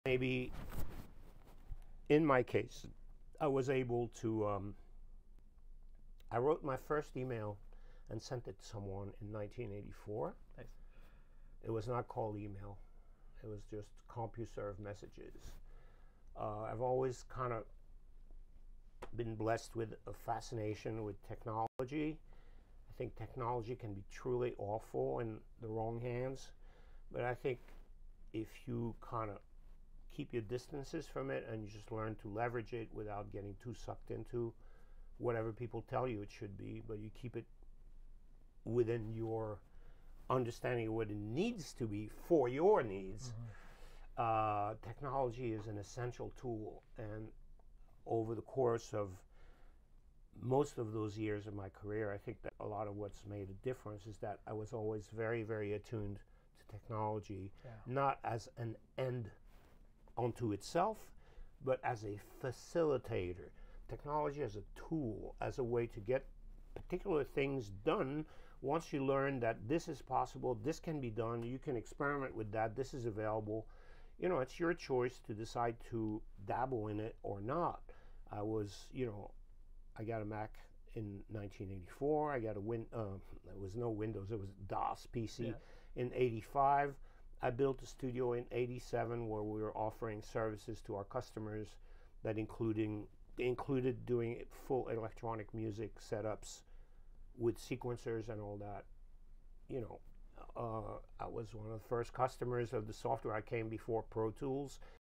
Embracing Technological Innovation for Success | François K on Hip Hop Hello
Francois K on his history with and use of technology.

"Maybe in my case I was able to. I wrote my first email and sent it someone in the year my It was not called email It was just CompuServe messages I've always kind been blessed with a fascination with technology I think technology can be truly awful in the wrong hands but I think if you kinda keep your distances from it and just learn to leverage it without getting too sucked into whatever people tell you it should be but you keep it within your understanding what it needs to be for your needs uh. technology is an essential tool and over the course of most of those years of my career i think that a lot of what's made a difference is that i was always very very attuned to technology not as an end unto itself but as a facilitator technology as a tool as a way to get particular things done once you learn that this is possible this can be done you can experiment with that this is available you know it's your choice to decide to dabble in it or not I was you know I got a Mac in Nineteen Eighty-Four I got a there was no Windows it was DOS PC in Eighty-Five I built a studio in Eighty-Seven where we were offering services to our customers that included doing full electronic music setups with sequencers and all that I was one of the first customers of the software came before Pro Tools."